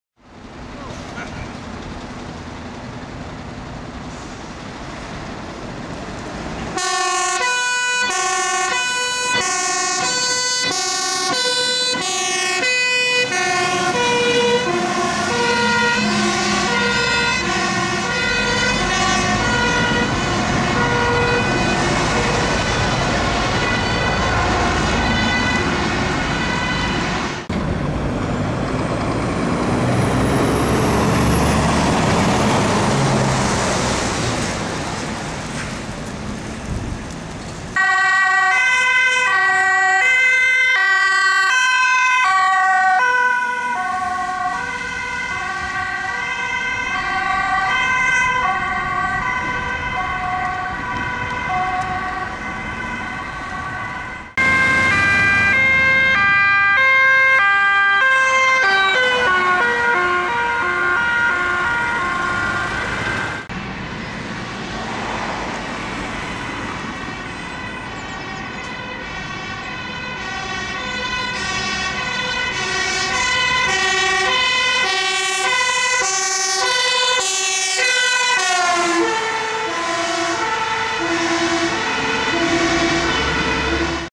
(Luchthoorn) Hulpdiensten met spoed naar een ongeval in Apeldoorn
©112Schiedam